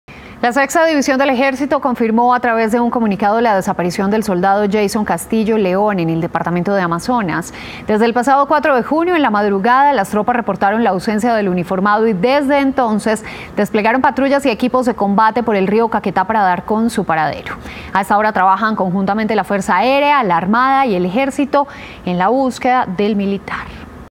Reportan soldado desaparecido en el Amazonas
Desde el pasado 4 de junio, tropas de la sexta división alertaron sobre la desaparición de Jeison Castillo León.